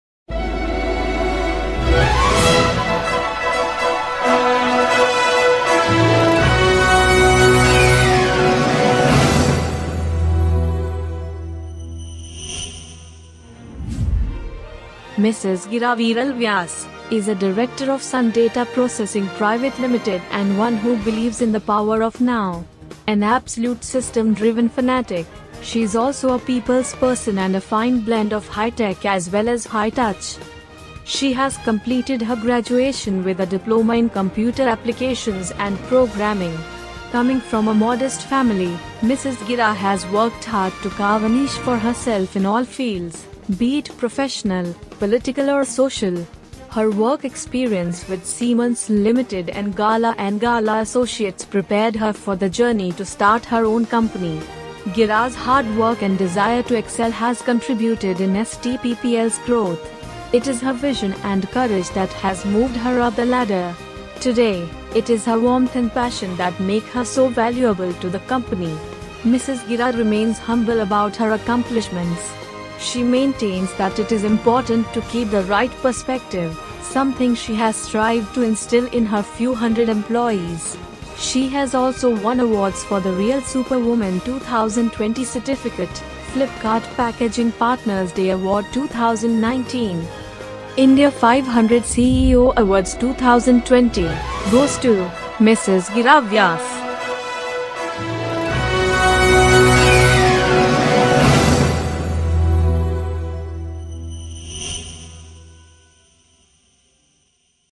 Mrs. Geera Vyas - Winner of India 500 CEO Awards 2020
Mrs. Geera Viral Vyas, is a Director of Sun Data Processing Pvt Ltd & one who believes in the power of ‘NOW’. An absolute system driven fanatic, she is also a people’s person and a fine blend of high ‘tech’ as well as high ‘touch’.

She has completed her graduation with a Diploma in Computer Applications & Programming. Coming from a modest family, Geera has worked hard to carve a niche for herself in all fields, be it professional, political or social. Her work experience with Siemens Ltd. and Gala & Gala Associates prepared her for the journey to start her own company. Geera’s hard work and desire to excel has contributed in SDPPL's growth. It is her vision and courage that has moved her up the ladder. Today, it is her warmth and passion that make her so valuable to the Company. Geera remains humble about her accomplishments. She maintains that it is important to keep the right perspective, something she has strived to instill in her few hundred employees. She has also won awards for The Real Superwoman 2020 Certificate, Flipkart Packaging Partner’s Day Award 2019.

India 500 CEO Awards goes to, Geera Vyas.